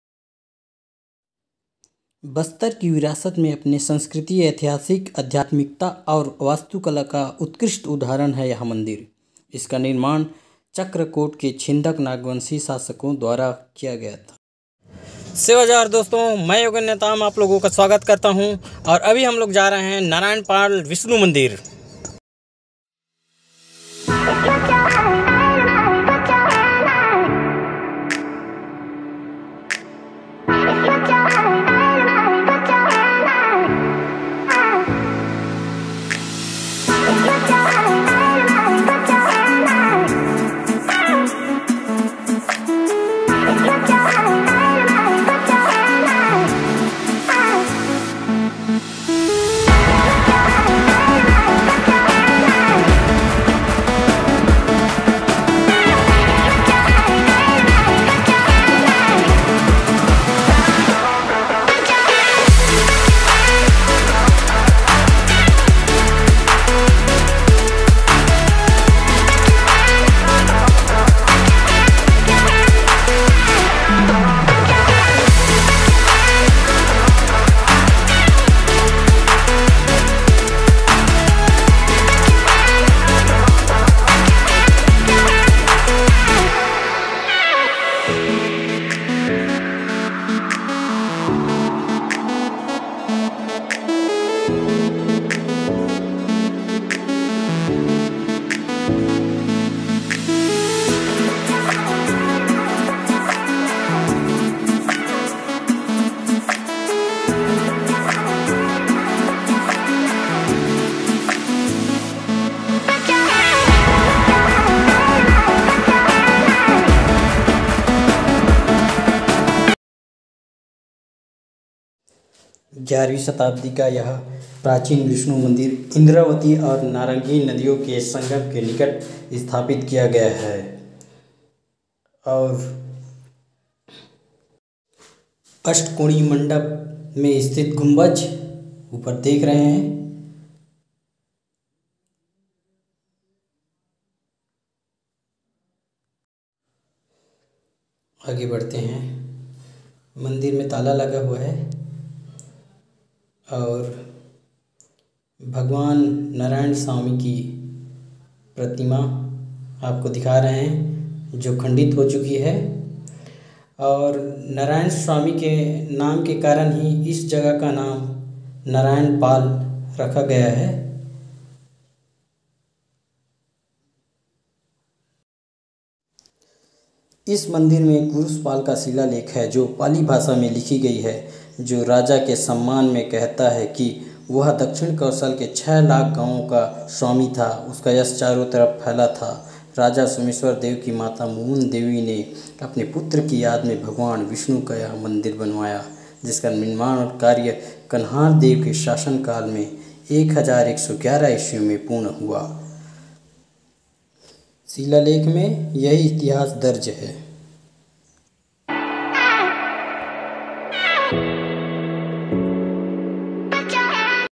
बस्तर के इतिहास का प्राचीन विष्णु मंदिर । ( नारायणपाल मंदिर ) vlogger - yogendra netam
Dosto,
          Mai yogendra netam mere channel me aap logo ka swagat hai.

नारायणपाल मंदिर -
                      ११वीं शताब्दी का यह प्राचीन विष्णु मंदिर इंद्रावती और नारंगी नदियों के संगम के निकट स्थापित किया गया है,  नारायणपाल मंदिर बस्तर की विरासत में अपने संस्कृति, ऐतिहासिक , आध्यात्मिक और वास्तुकला का उत्कृष्ट उदाहरण है । इसका निर्माण चक्रकोट के छिंदक नागवंशी शासकों द्वारा किया गया।
                 इस मंदिर में कुरूषपाल का शिलालेख है, जो पालि भाषा में लिखी गई है जो राजा के सम्मान में कहता है कि, वह दक्षिण कौशल के छः लाख गांवों का स्वामी था , उसका यश चारों तरफ फैला था ।  राजा सोमेश्वर देव की माता मुमुंददेवी ने अपने पुत्र की याद में " भगवान विष्णु " का मंदिर बनवाया । जिसका निर्माण कार्य कन्हारदेव ( मुमुंददेवी का पौत्र ) के शासन काल ११११ ई. में पूर्ण हुआ । मंदिर के अष्टकोणीय मंड़प में स्थित शिलालेख में यही इतिहास दर्ज है ।
                  यह मंदिर वस्तुत: शिव मंदिर था  किन्तु परवर्तीकाल में  इस मंदिर में भगवान विष्णु की प्रतिमा स्थापित किया गया । और इस तरह से इस जगह का नाम  नारायणपाल पड़ा ।

Location -  Narayanpal temple located approximately 35 km to the North - West  side of Jagdalpur , in Bastar district in the Indian State of Chhattisgarh .

Video - mobile camera
vlog - 3
friends - Prashant, Suresh, Vicky